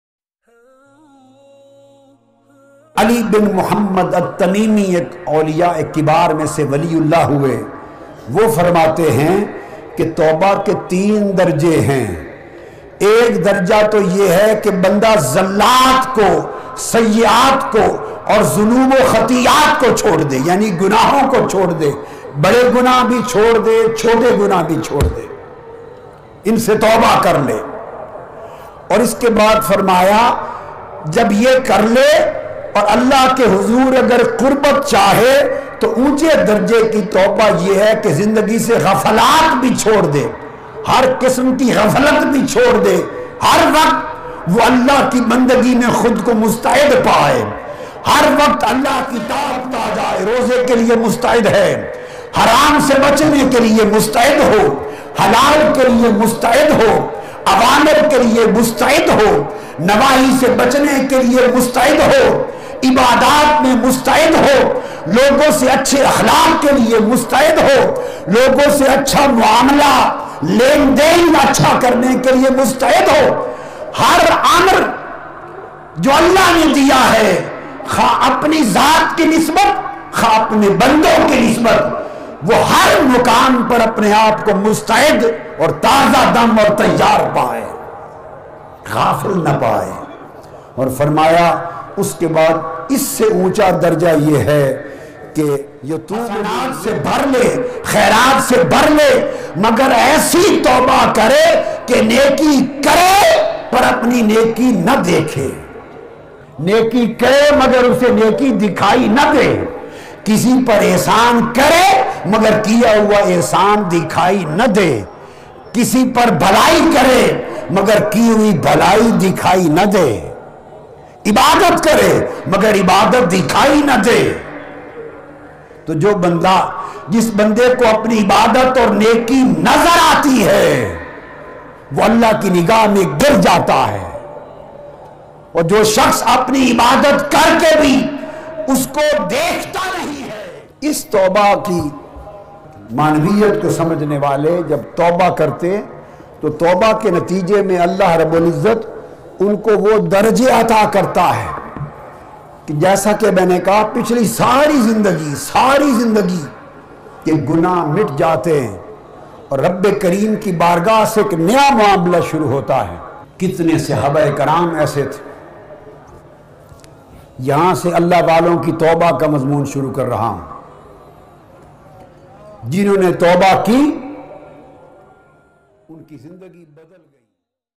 Toba kese karen ll Tauba karne ke yareeqe aur darjaat ll Tahir-ul-qadri speech ll Bazurgane Deen
0:00 tauba kese kare
1:00 Tauba ke darjaat
2:00 Tahir-ul-qadri speech
powerd by Bazurgane Deen